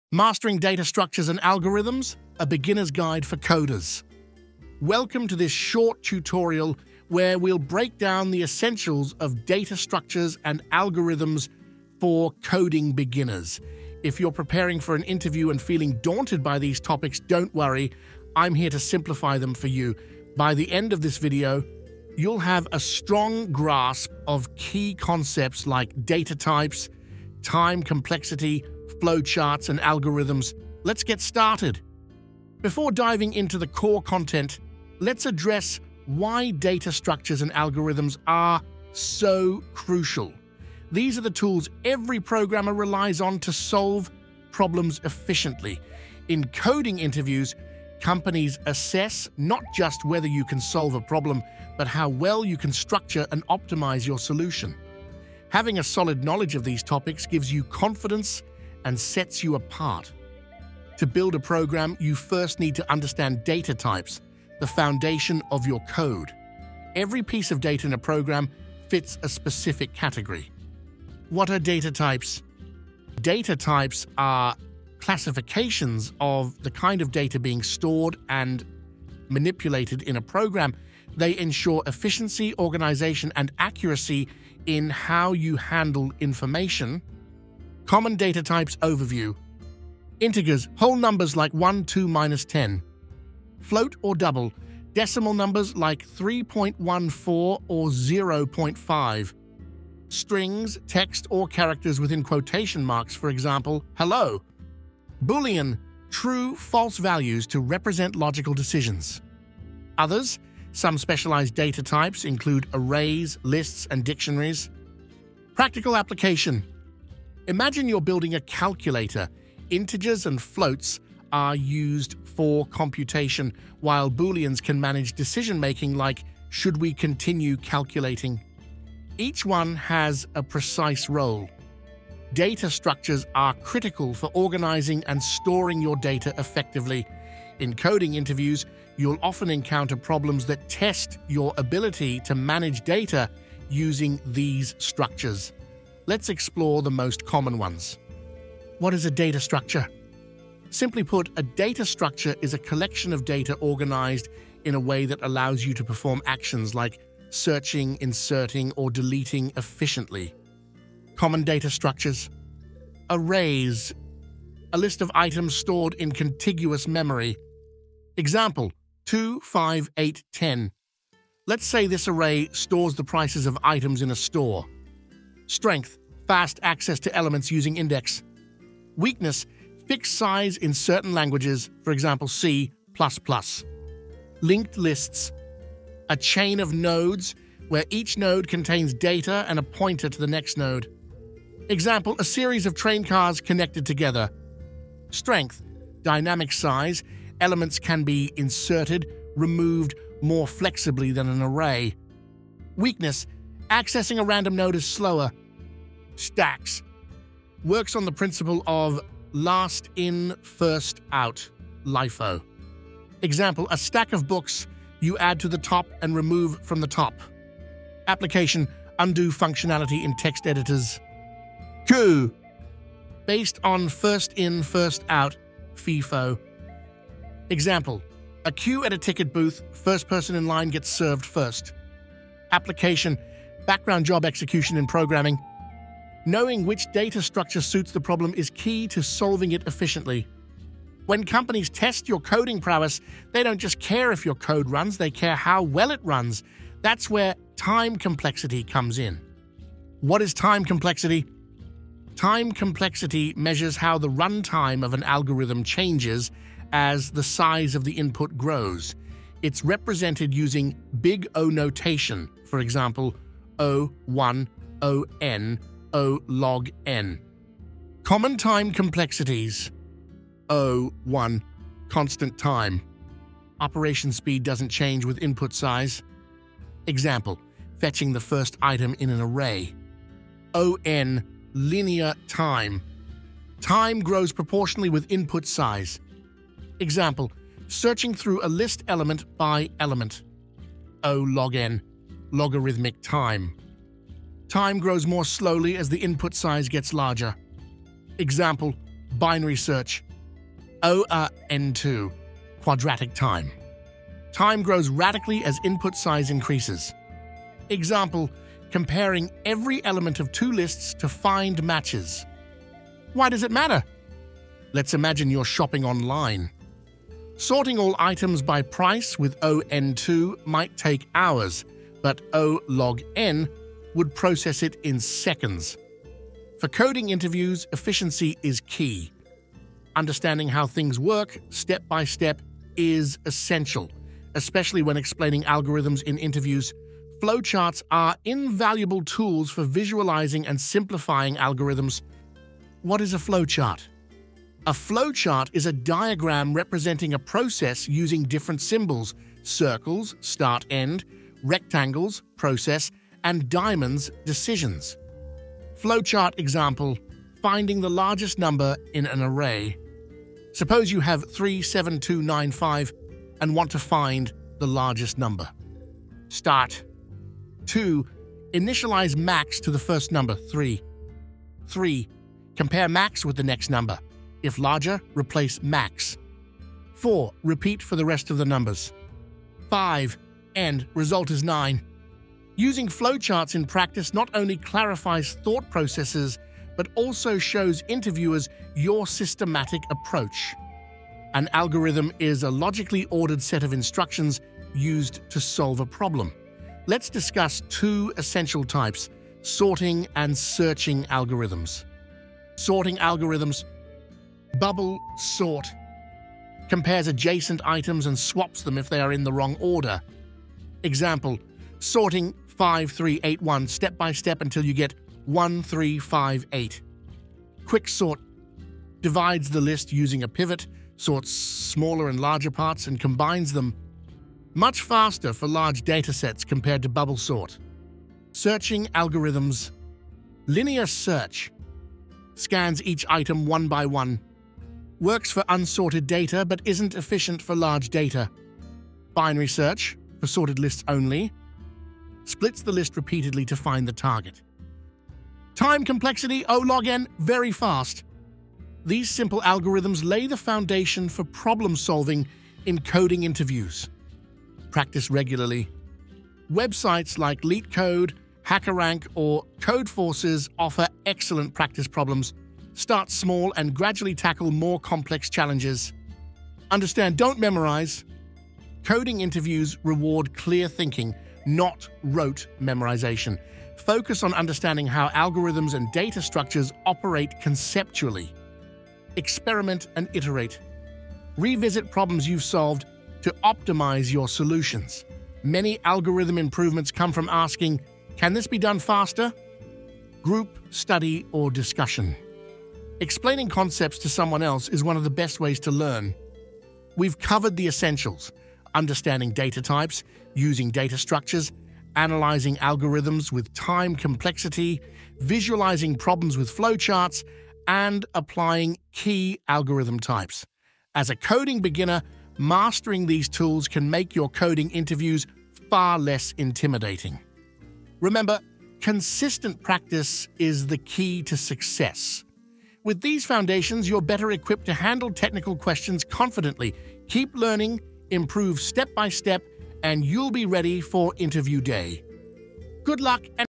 🚀 Mastering Data Structures and Algorithms: A Beginner’s Guide for Coders 🚀
Unlock the secrets of programming excellence! This video is your ultimate beginner's guide to understanding Data Structures and Algorithms (DSA). Whether you’re preparing for coding interviews or just starting your coding journey, this guide breaks down complex concepts into simple, actionable steps.

🧠 What you'll learn:
✅ What are Data Structures and Algorithms?
✅ Key types: Arrays, Linked Lists, Stacks, Queues, Trees, Graphs, and more.
✅ Sorting & Searching algorithms explained (Bubble Sort, Merge Sort, Binary Search, etc.)
✅ Real-world applications of DSA concepts.
✅ Tips for cracking coding interviews!

📌 Why DSA is crucial:
Mastering DSA enhances your problem-solving skills, optimizes your code, and makes you stand out in tech interviews.

🔗Practice platforms: LeetCode, HackerRank, and Codeforces

💬 Have questions? Drop them in the comments, and I’ll answer them!